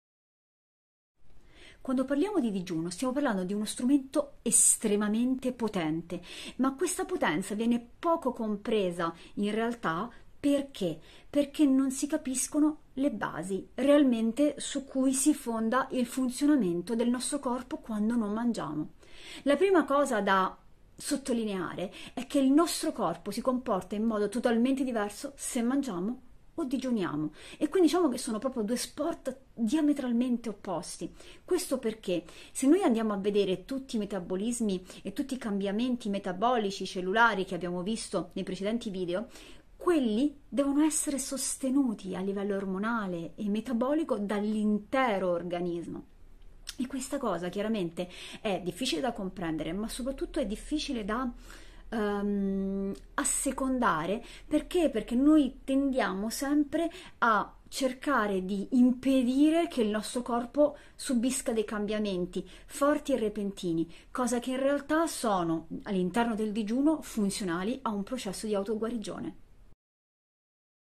LE RIVOLUZIONI DEL DIGIUNO
Il digiuno stravolge il corpo?
E noi dobbiamo sostenerlo... Non impedirlo.
Tutti i cambiamenti fisiologici che avvengono durante il digiuno sono la base dell'auto-guarigione.

Se li blocchiamo impediamo al corpo di guarire.